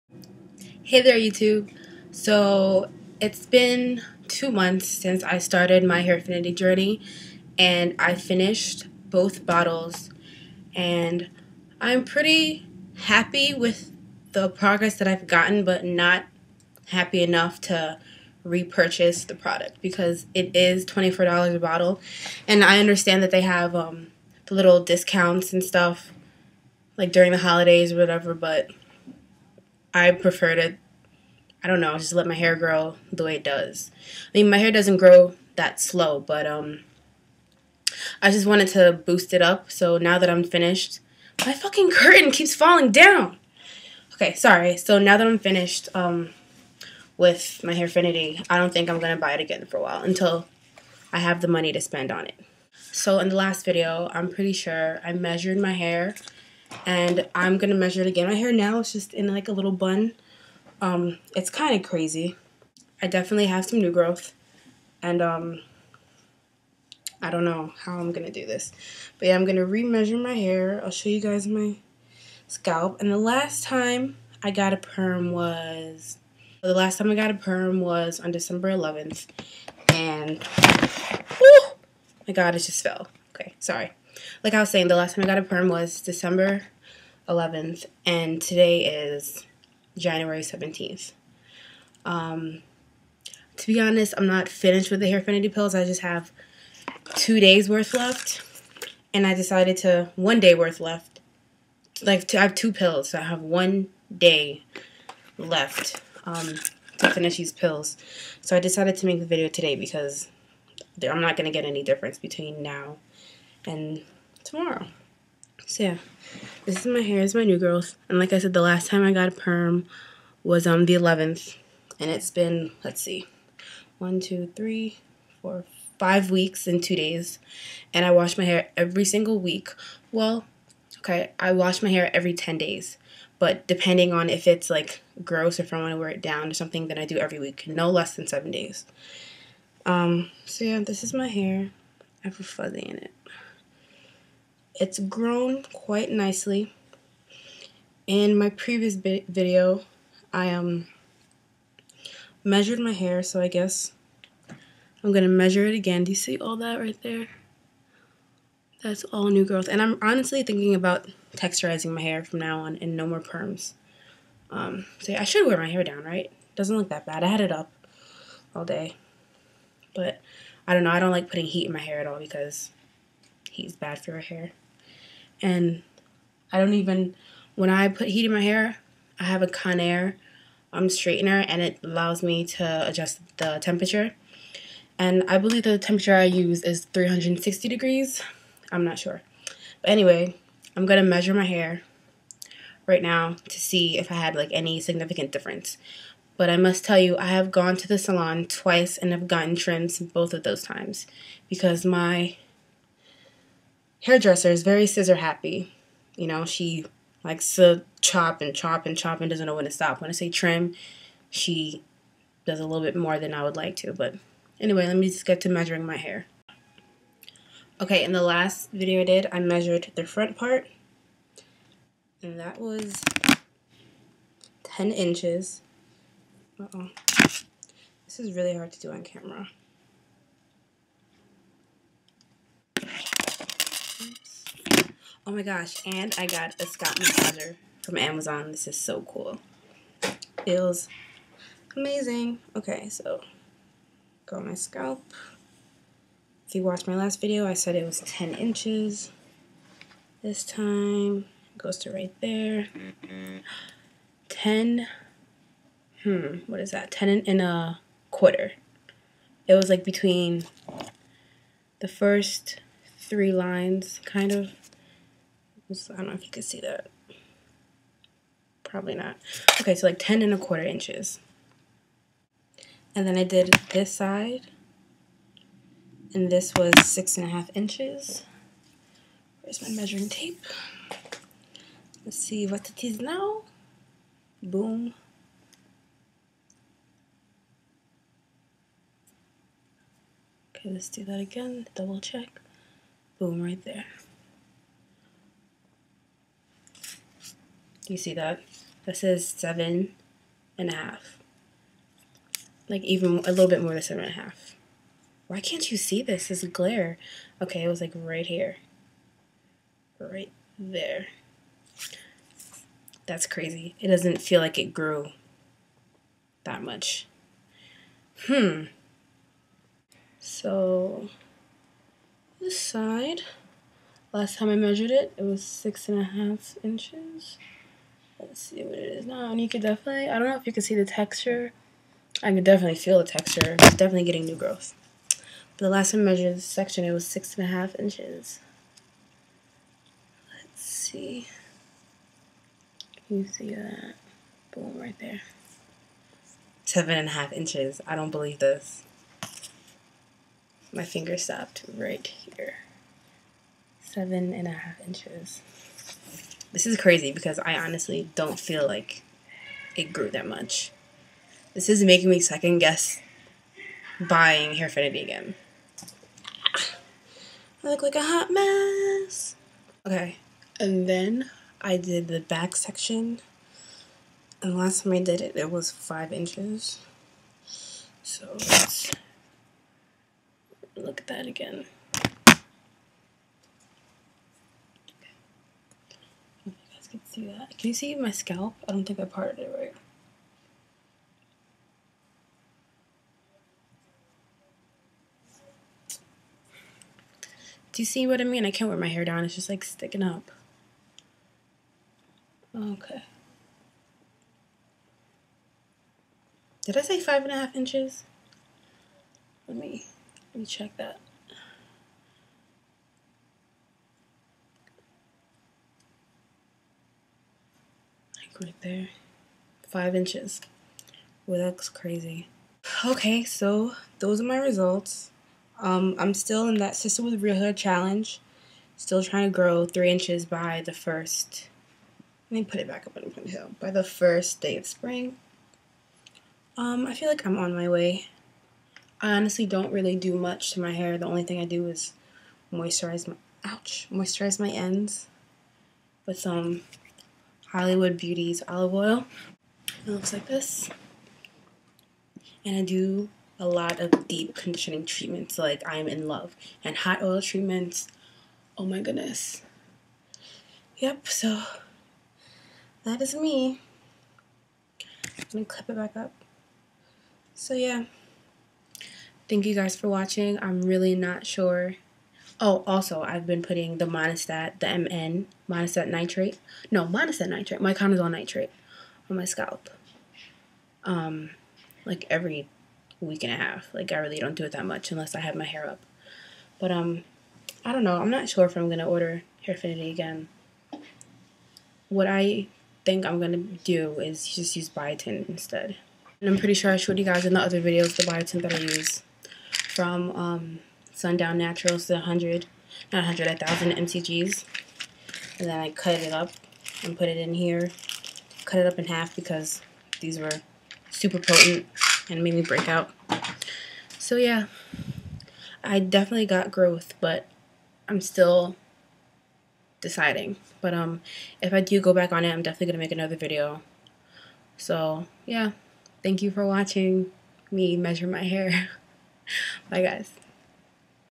Hairfinity 2 Month Update and Length Check
Hey Guys,

So in this video I am basically talking to you guys about Hairfinity and how it worked for me. I also forgot to mention in the video that the headaches that I used to get in the beginning went away after a couple days, and the shedding slowed down as well.